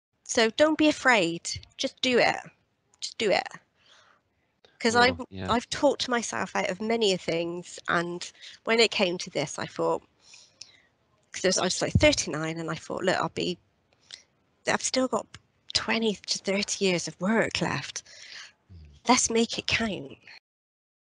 NHS Project Futures Apprenticeships - Emily - "Just Do It!" *Soundbite
If you’re curious and either a potential learner or line manager, now’s the time to find out more, simply email Mike Gray at